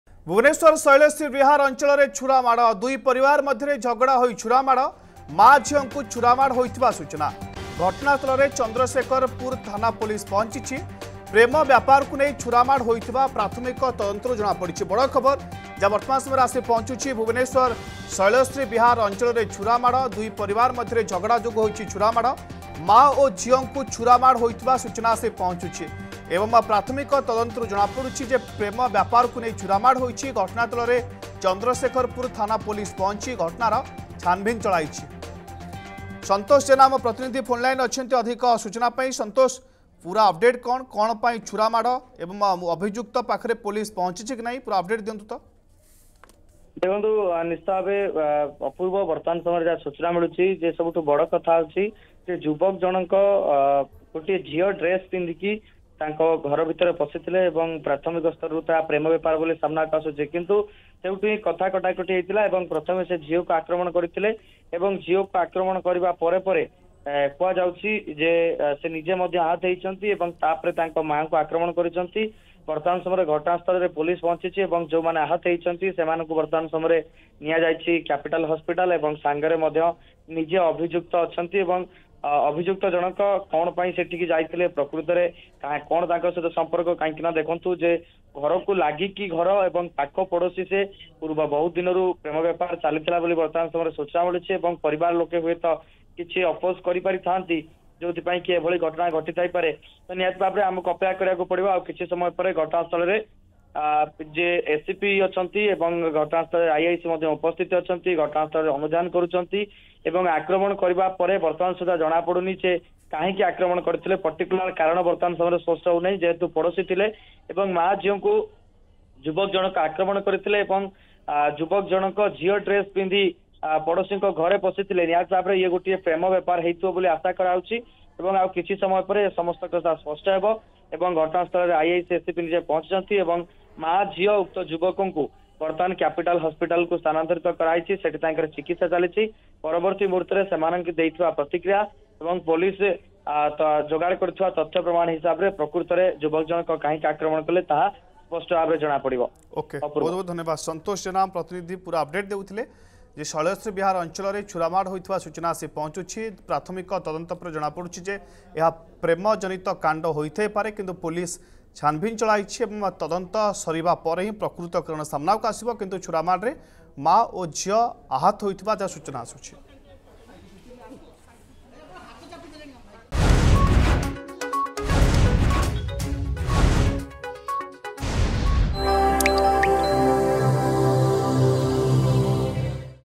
ରାଜଧାନୀରେ ଦିନ ଦ୍ବିପହରରେ ଛୁରାମାଡ଼
ଭୁବନେଶ୍ୱର ଶୈଳଶ୍ରୀ ବିହାର ଅଂଚଳରେ ଛୁରମାଡ଼ । ଦୁଇ ପରିବାର ମଧ୍ୟରେ ଝଗଡା ହୋଇ ଛୁରା ମାଡ଼ । ମା' ଝିଅ ଙ୍କୁ ଛୁରା ମାଡ଼ ହୋଇଥିବା ସୁଚନା ।